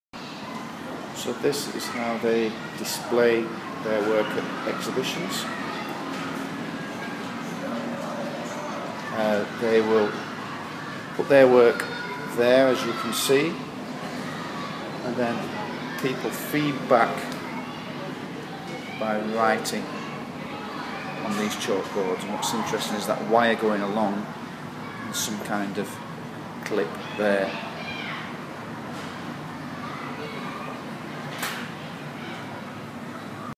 HTH Critique / AfL in Corridors
Video from our visit to High Tech High, San Diego, USA. Clip shows how space is used to support learning.